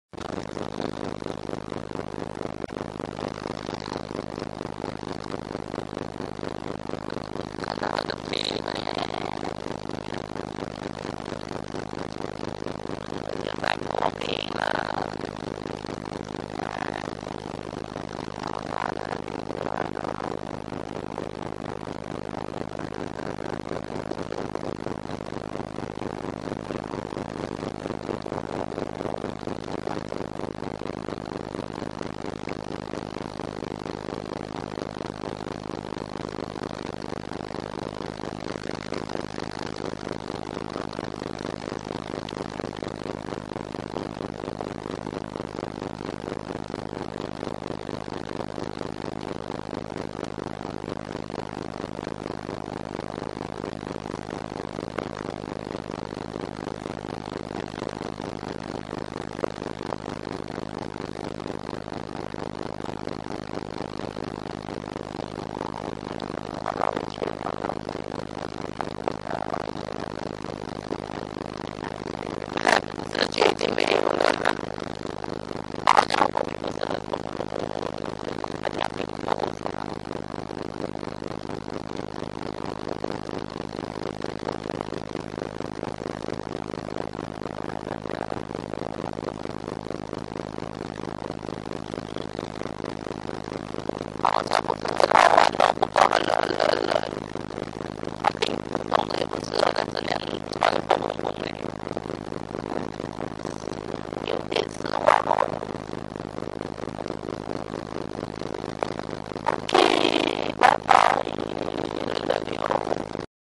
Ice breaking tour
See how the ship sails across the Hokkaido's sea? The ice needs to be thick enough for that.